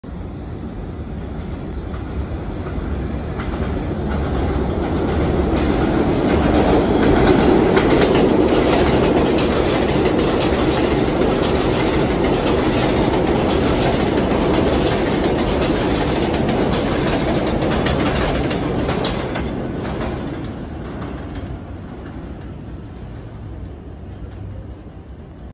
Wakefield Bound R142 5 Train Bypassing Burke Avenue
Taken October 30, 2022

Because of construction, Like the G.O that happened in January, March, April, and today 2/5 Trains were running as follows

2. Eastchester-Dyre Avenue (on the 5 Line) to 34th Street then ran Local to South Ferry 1 station)
5. Wakefield-241st Street (on the 2 Line) to Flatbush Avenue Via Lexington Avenue and loacl service in Brooklyn and Bronx (Besides Manhattan Bound in Brooklyn)

Seen here is a Wakefield Bound R142 5 Train Bypassing Burke Avenue. That reasn was because there was other 5 trains behind to the point there was 5 trains running on the Express track from East 180th Street to Gun Hill Road